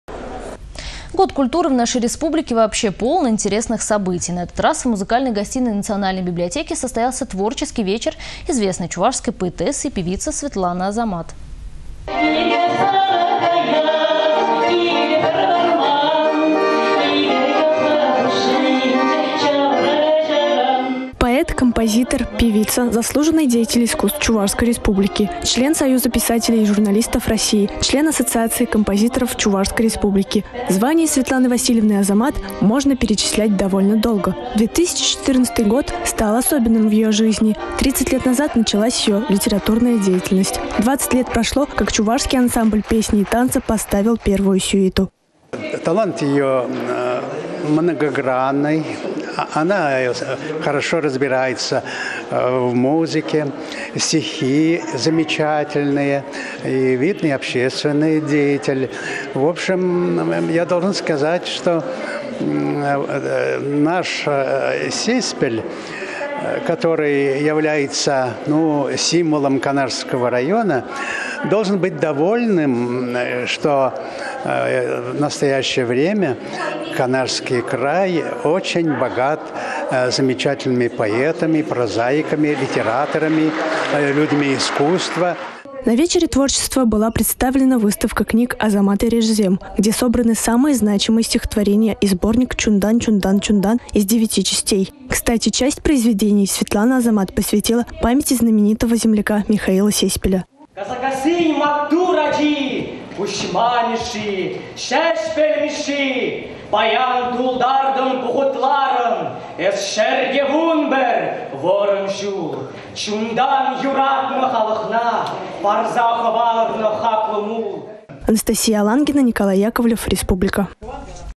В музыкальной гостиной Национальной библиотеки состоялся творческий вечер Светланы Азамат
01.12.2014
Год культуры в нашей республике полон интересных событий. На этот раз в музыкальной гостиной  Национальной библиотеки состоялся творческий вечер известной чувашской  поэтессы и певицы Светланы Азамат.